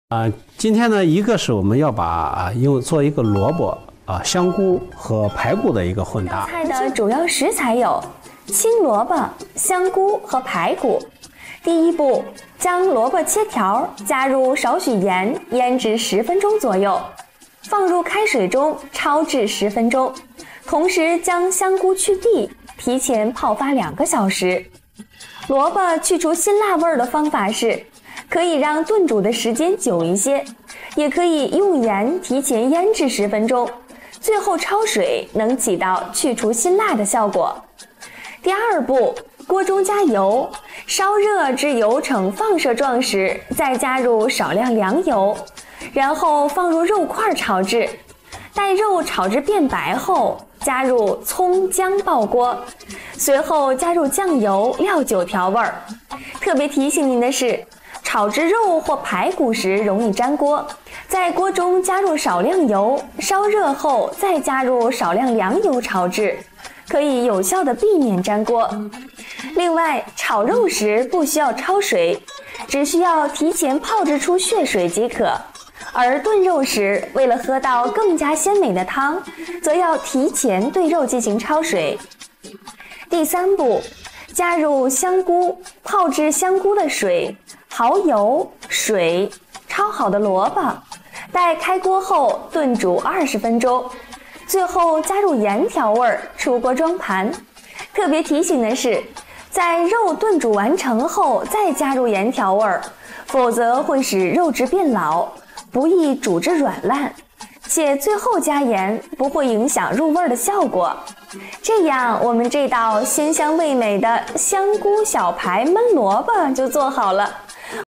【中華美食屋】兩根排骨，半碗香菇，半根蘿蔔！大廚教您做香菇排骨燜蘿蔔，好吃又饞嘴,太香了！
匯聚知名權威營養專家及烹飪大師！
以教授美食烹飪技藝為主！
穿插大量健康營養方面的實用知識！
烹飪技巧簡單易學！
歡迎大家收看！